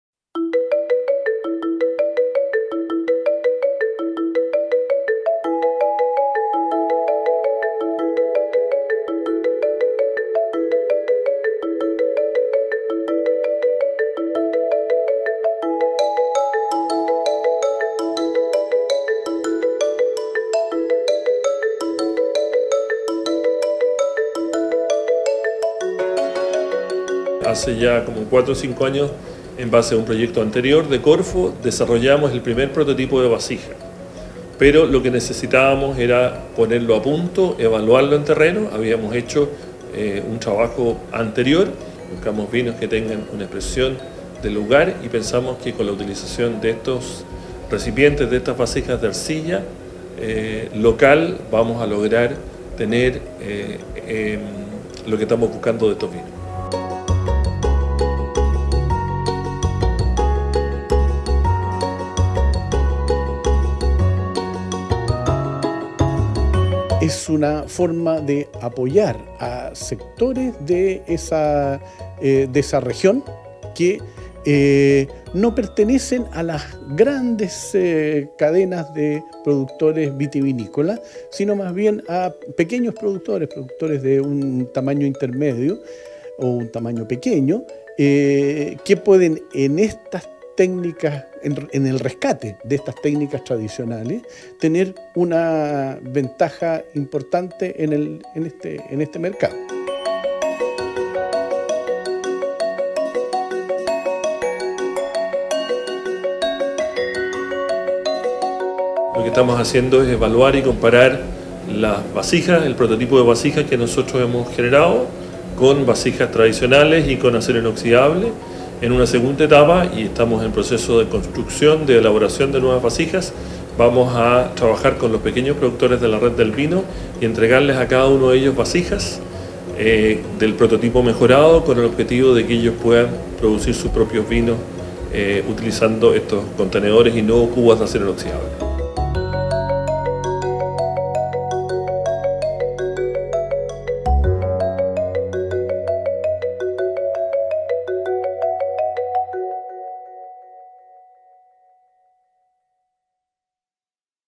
Proyecto FIC-R 05 Vasijas
Evaluación de un prototipo de vasijas de vinificación y guarda fabricadas con arcillas de Pañul, para pequeños productores vitivinícolas de la región de O'Higgins.